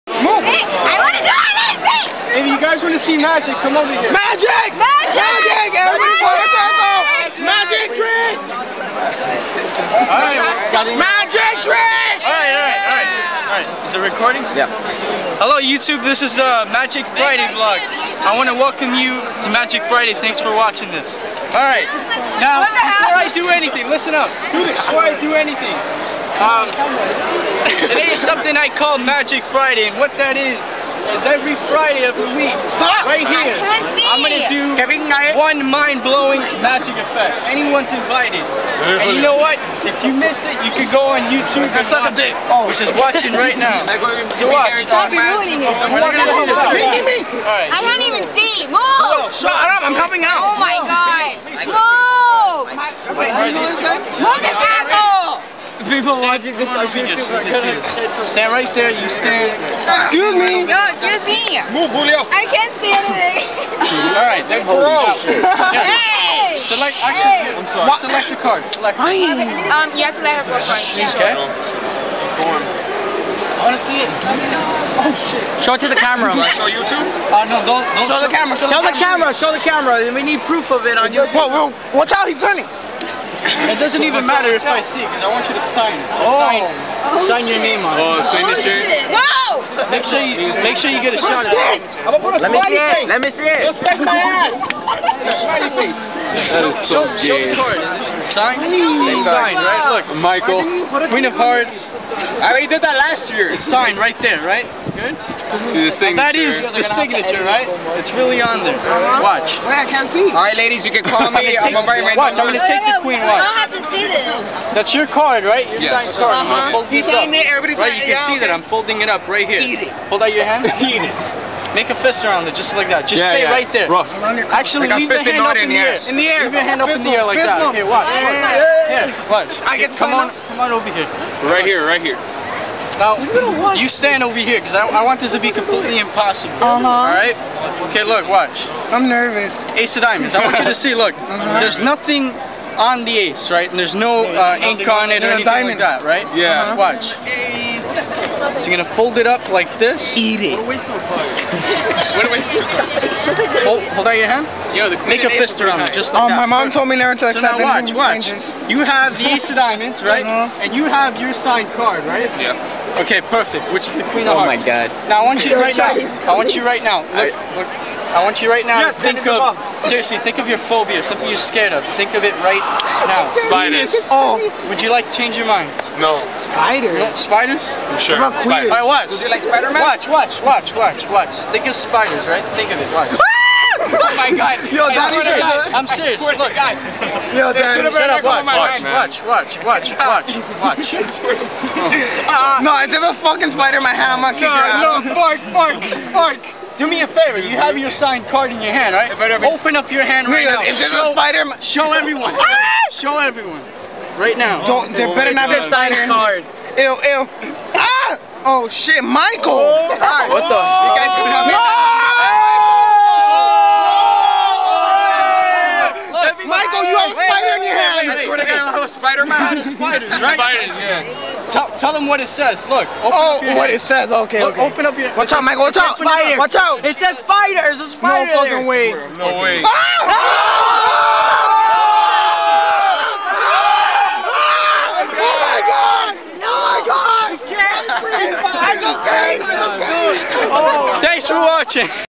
Magic Friday Vlog: Day 1
Day 1 of magic friday vlog. If anyone thinks this is a camera trick then you guessed wrong because it's not. If you see one of the people in the video ask them in person and they will tell you that what you see is what you get.